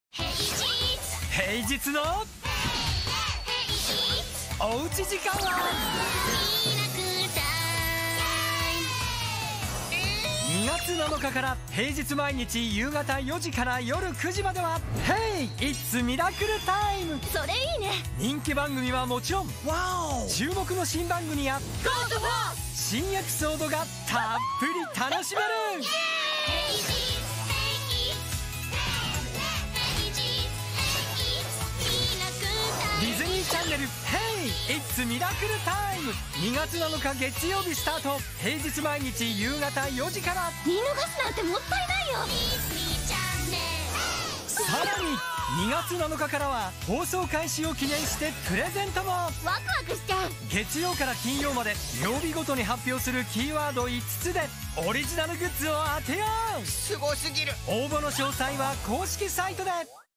Disney Channel Japan: Hey! It's Miracle Time Promo
This February 7th Disney Channel Japan had various new shows and episodes starting at 16:00 with the promotional video "Hey! It's Miracle Time".

© This channel is had copyrighted content, some of it will inevitably be the work of others.